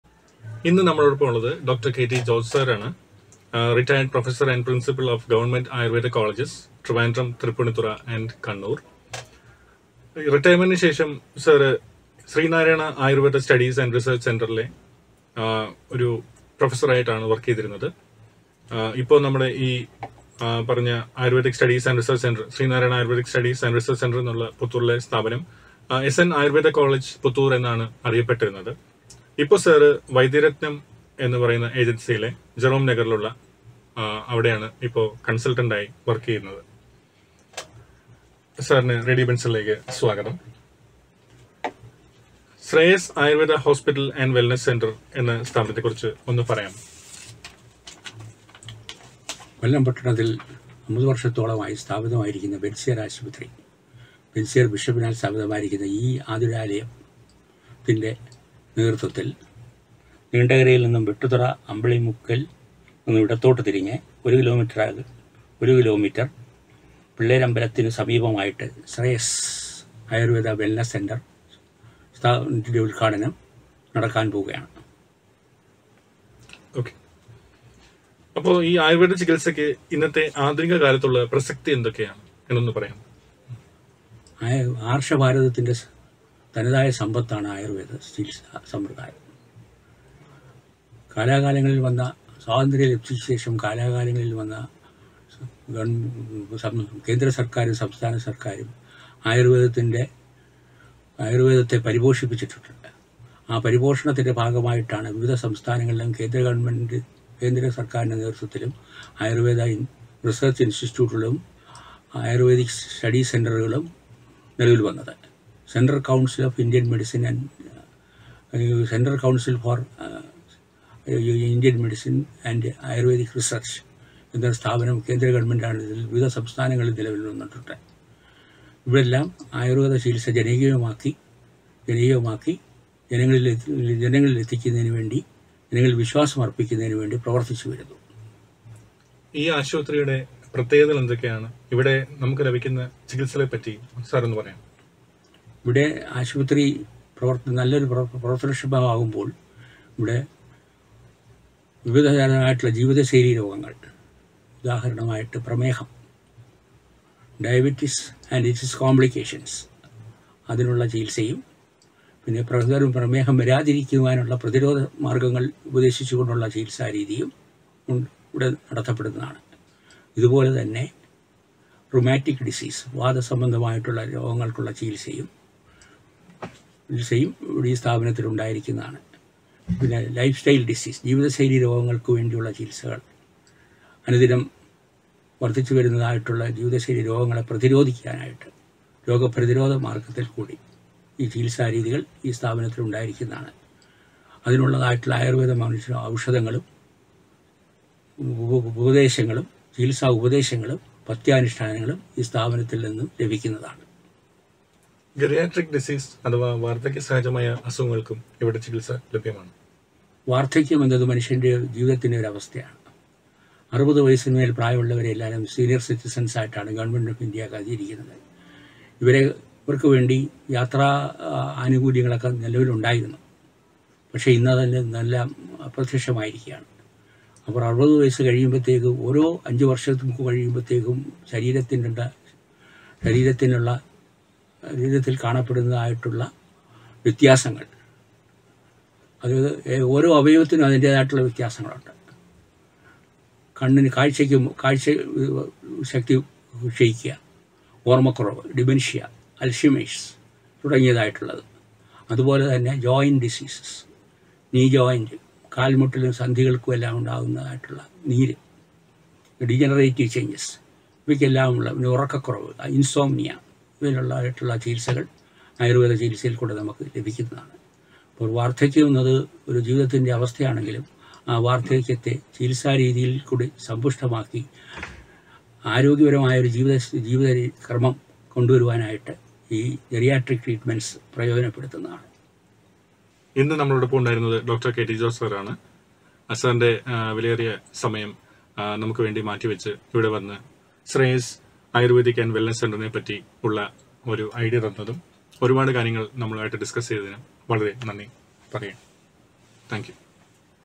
ശ്രേയസ് ആയുർവേദിക് ആൻഡ് വെല്‍നെസ്സ് സെന്റര്‍ | Interview | Community Radio Benziger 107.8
ശ്രേയസ്  ആയുർവേദിക്  ആൻഡ്  വെല്‍നെസ്സ് സെന്റര്‍ | Interview | R J Mahesh Madhu | Community Radio Benziger 107.8